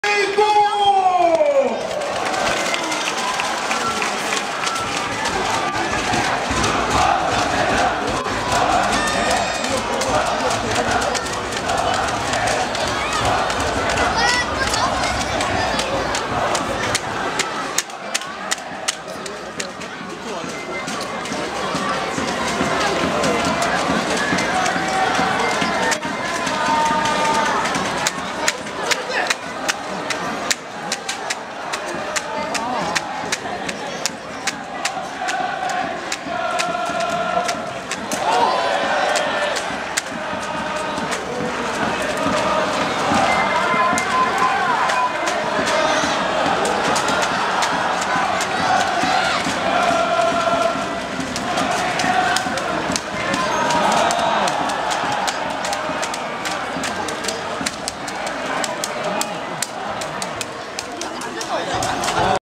21#160312토.리그개막전 성남vs수원직캠#21
160312토.리그개막전 성남vs수원직캠#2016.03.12.토 14:00 @탄천종합운동장 2016 K리그 클래식 개막전 성남vs수원/재업로드는 절대 하지 말아 주세요 부탁드립니다.퍼가실때 출처꼭 밝혀주세요!상업적 이용을 절대 금지합니다.감사합니다!페이스북:VAMOS EKDMZ.알싸 국톡방 수원★클래식_닥마 / 페이스북 페이지: "ULTRAS COREA. 12번째 선수들"